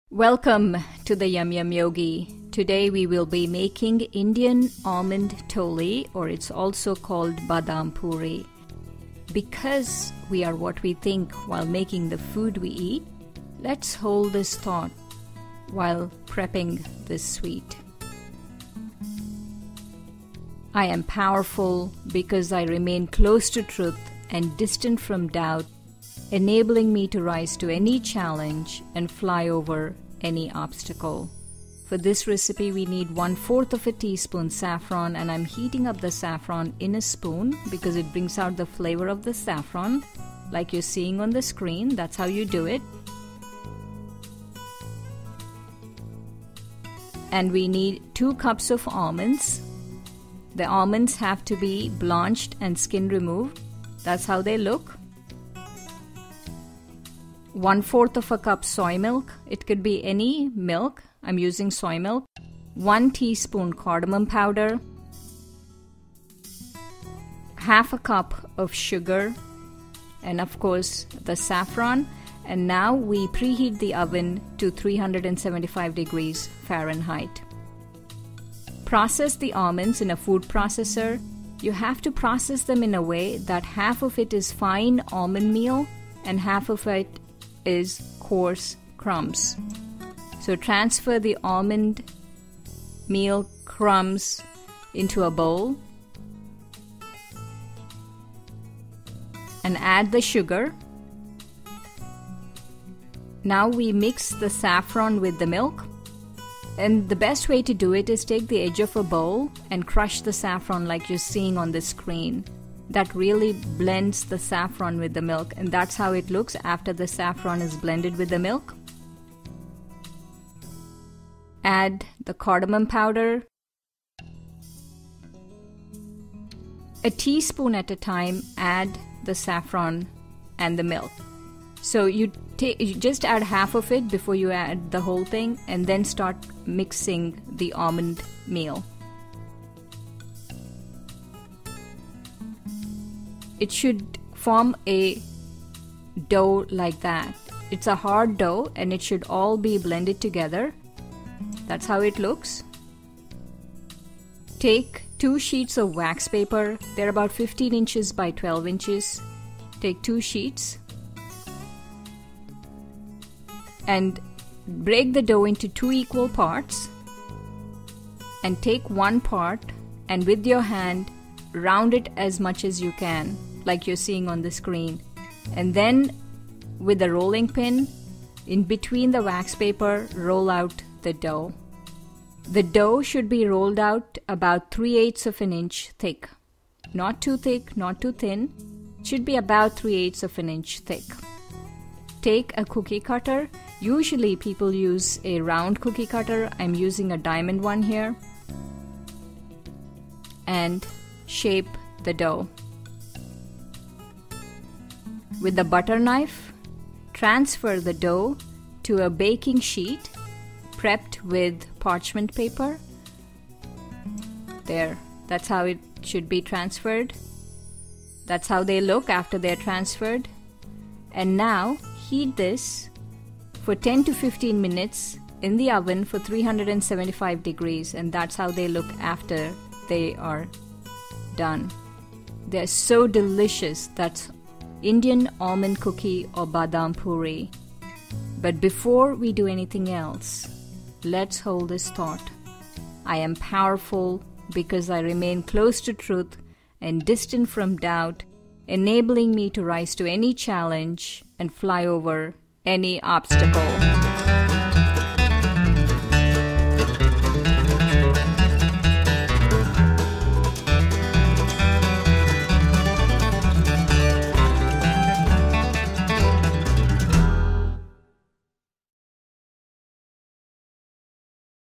Indian Almond Cookie (Badam Puri)
This gluten free cookie is a light, low sugar snack for your tea time or anytime.  I dare you to have just one!